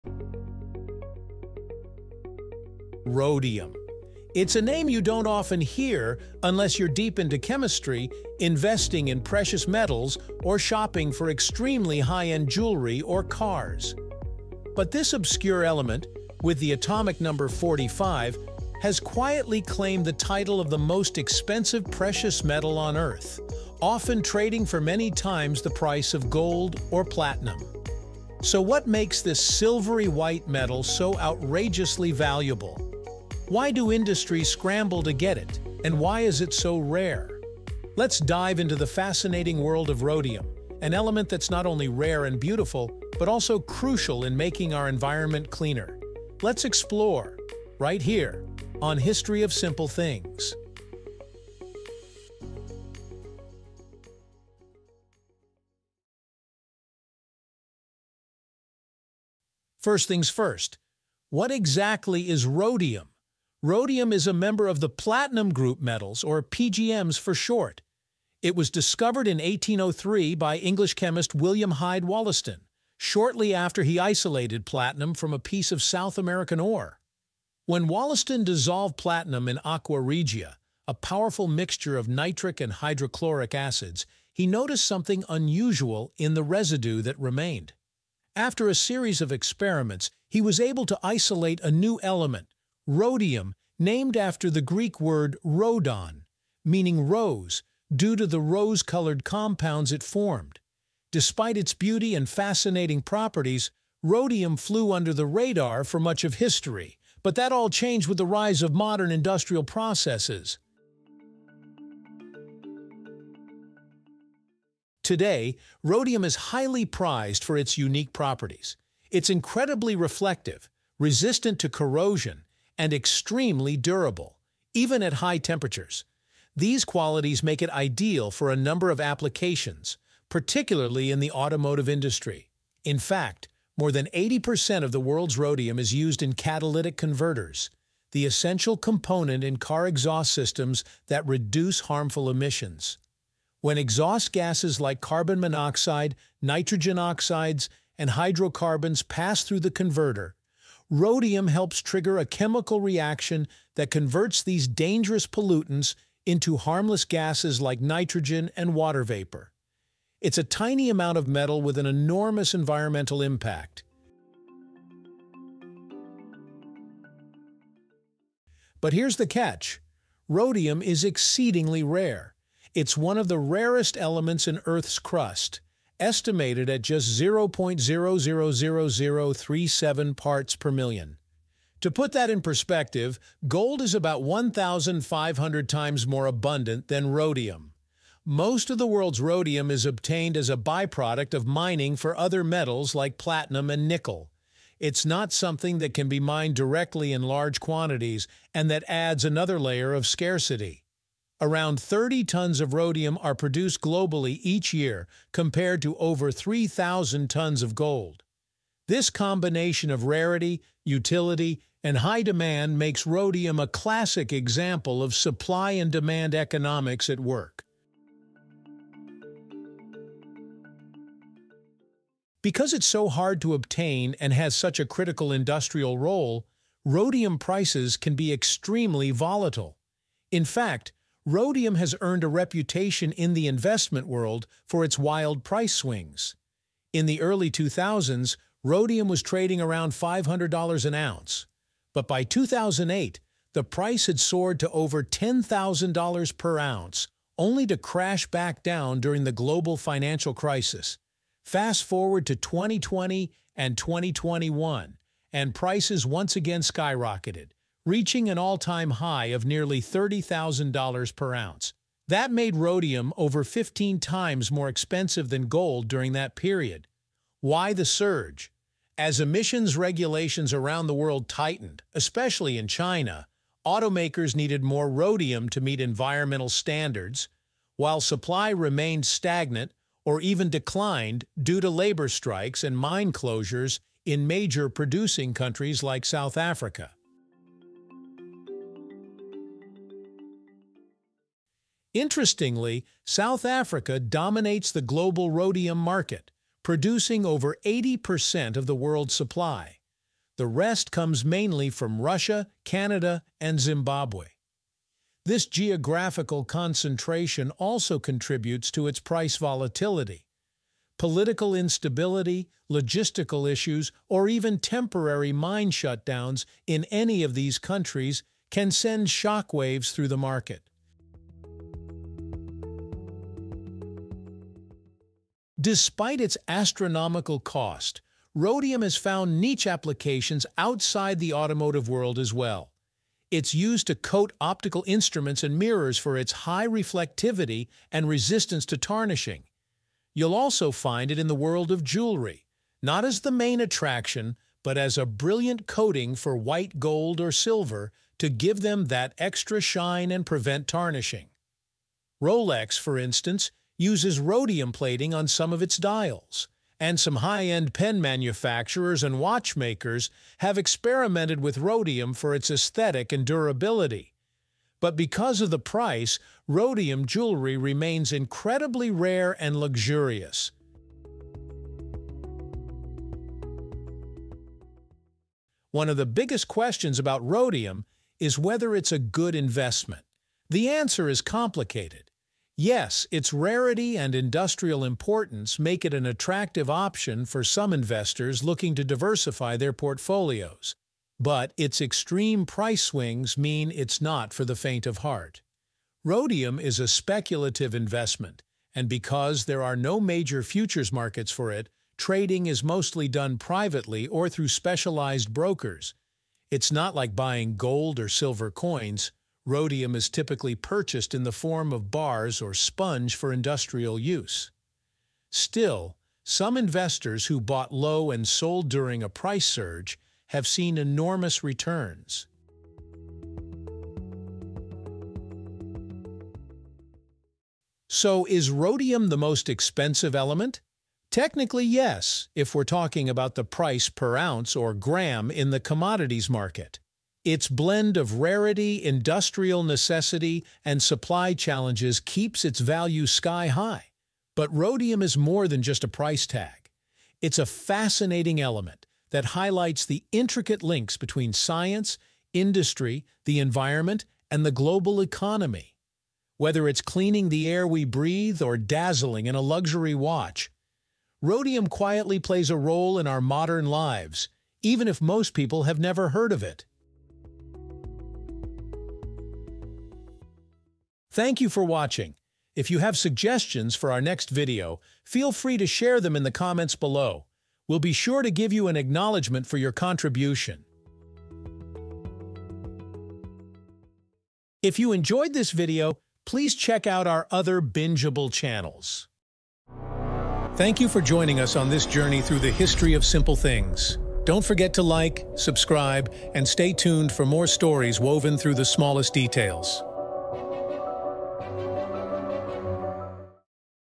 Why Is Rhodium So Expensive? The World’s Rarest Precious Metal Explained
Why is this little-known metal worth more than gold, platinum, or diamonds? In this video, we explore the fascinating story of rhodium—from its discovery to its critical role in modern technology, and why it commands such a jaw-dropping price.

Whether you’re into chemistry, precious metals investing, or just love learning about rare elements, this deep dive will explain everything you need to know about the world’s most valuable element.

⏱ Timestamps:
0:03 – Intro: What is the most expensive element?
0:59 – The discovery of rhodium
1:54 – Rhodium's unique properties
2:48 – Why rhodium is used in catalytic converters
3:42 – How rare is rhodium?
4:07 – Rhodium price history and volatility
4:51 – Where rhodium is mined
5:25 – Rhodium in jewelry and luxury products
6:15 – Should you invest in rhodium?
7:08 – Conclusion: Why rhodium’s value goes beyond price